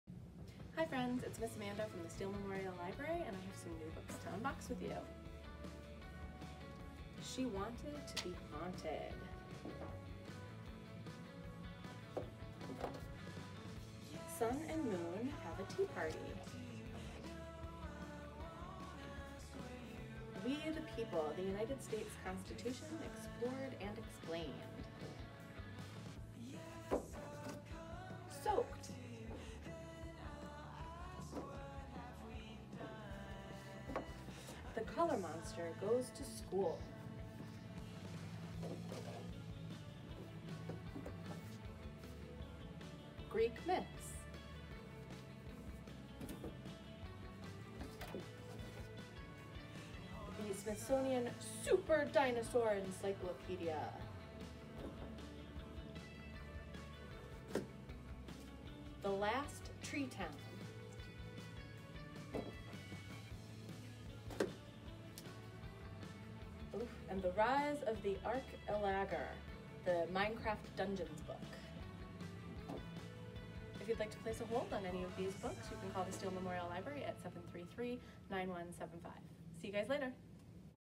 New children’s books at Steele Memorial Library, part 23.
Join Ms. Amanda as she unboxes new children’s books being delivered to the Steele Memorial Library.

Want to place a hold for curbside pickup at the Steele Memorial Library?